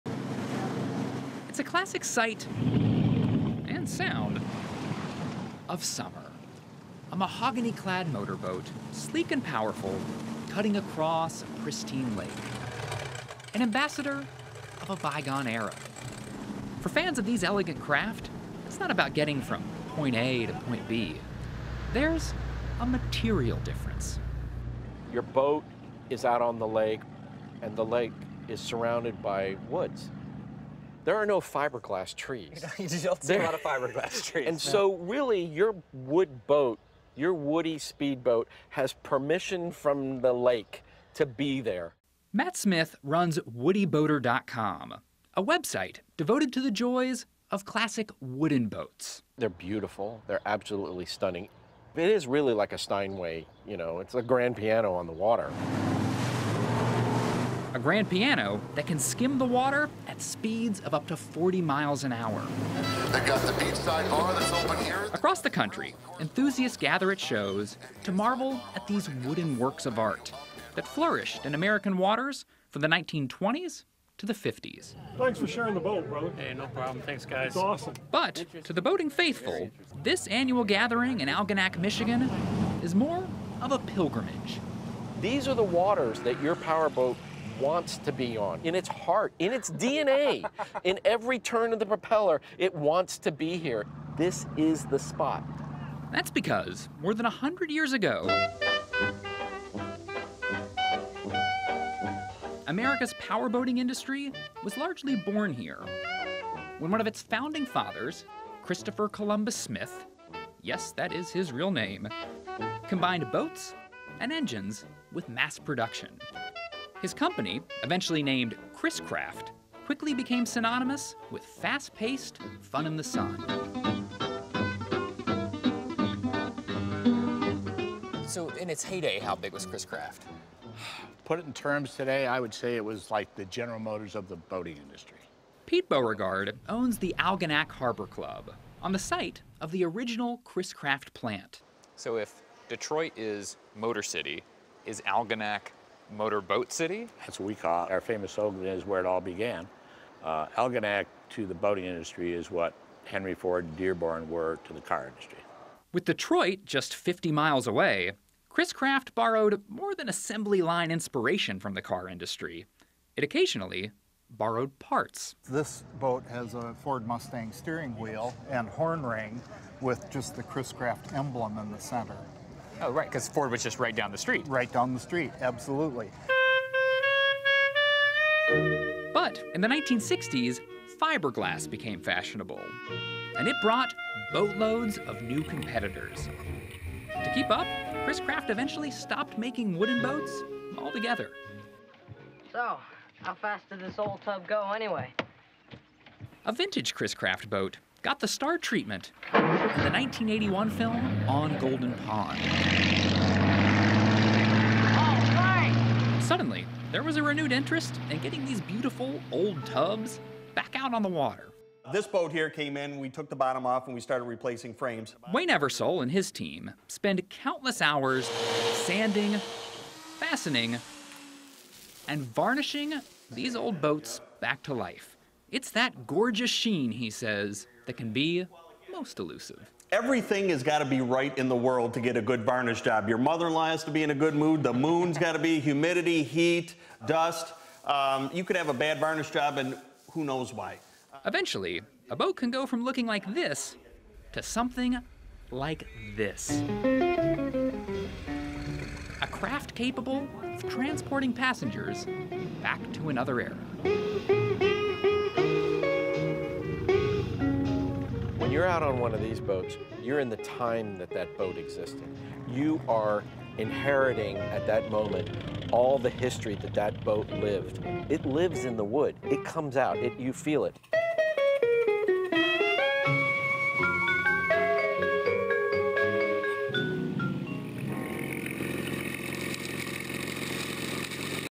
The elegance of classic wooden motorboats
Conor Knighton takes us on a ride aboard some classic wooden motorboats, whose sleek lines and elegant fixtures hark back to a golden era of maritime recreation.